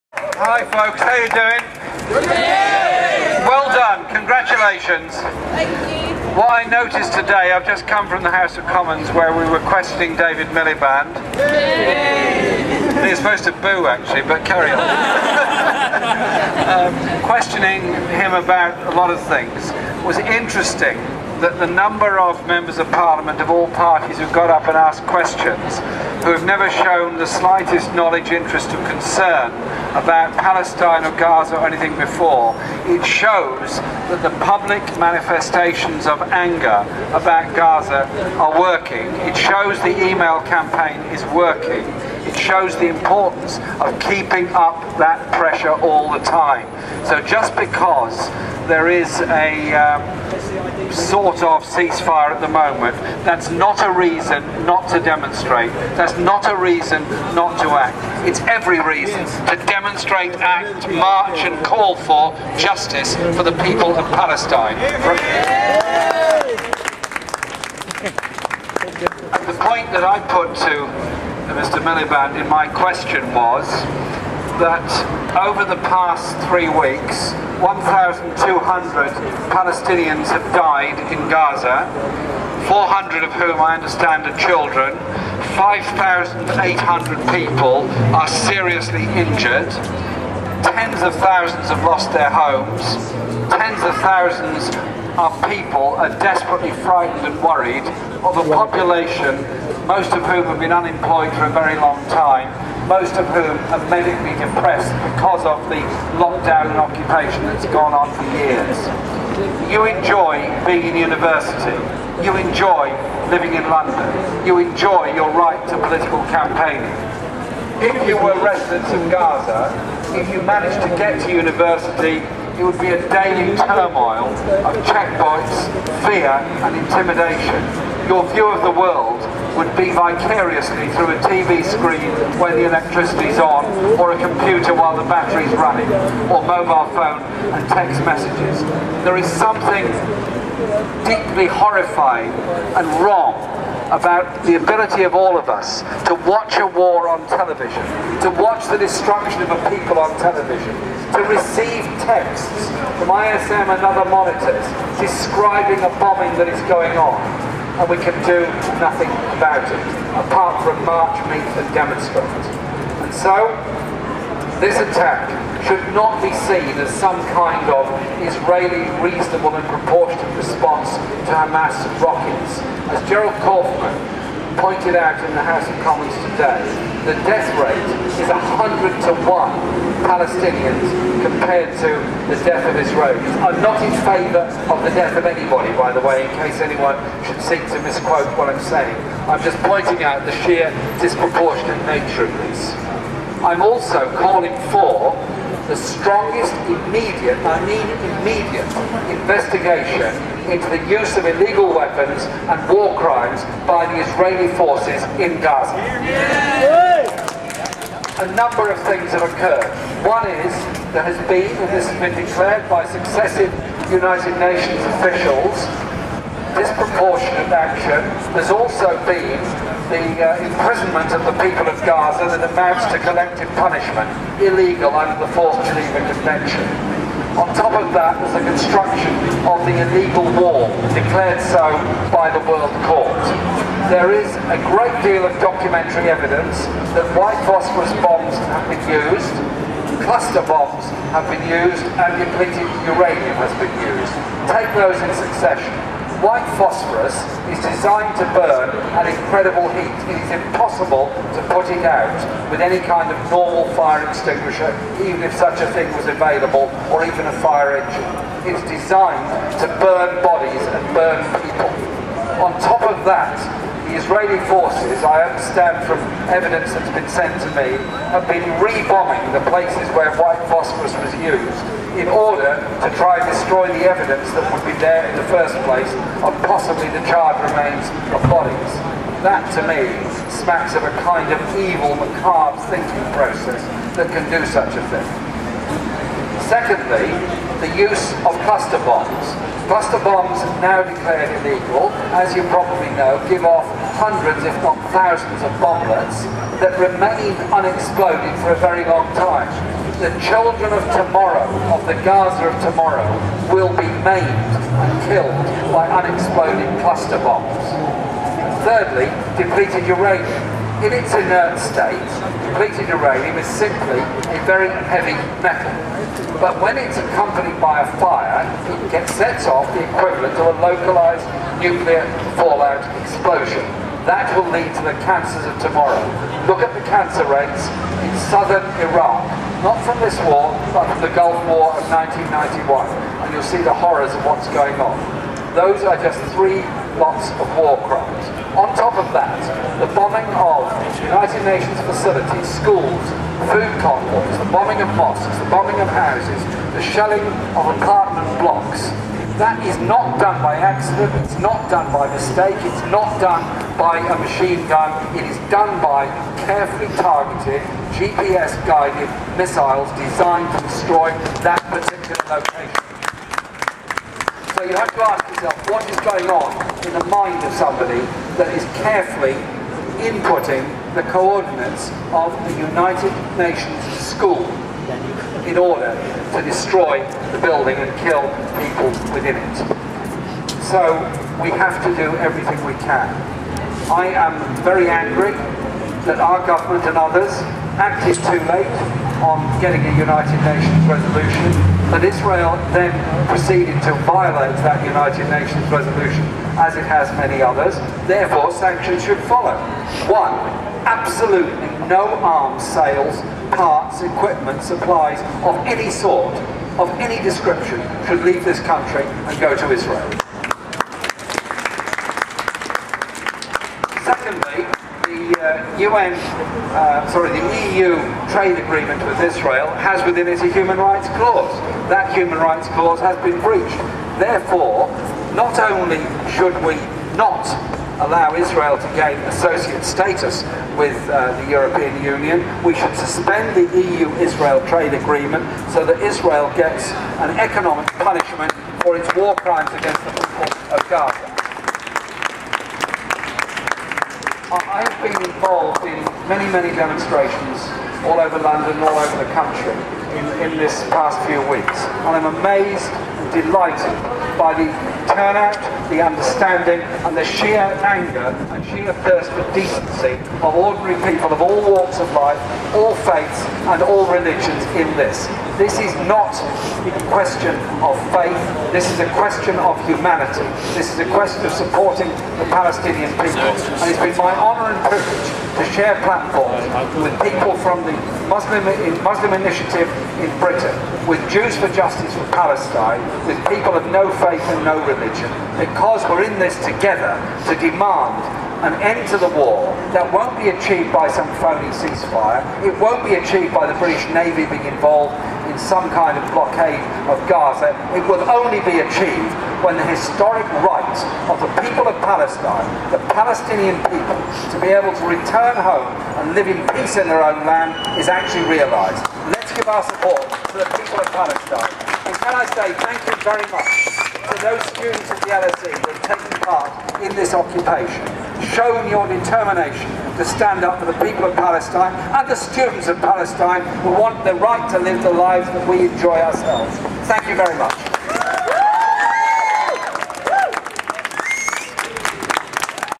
Jeremy Corbyn MP supports the occupation
80 students are occupying a lecture theatre at the London School of Economics. On the evening of the fifth day Jeremy Corbyn MP came to express his support.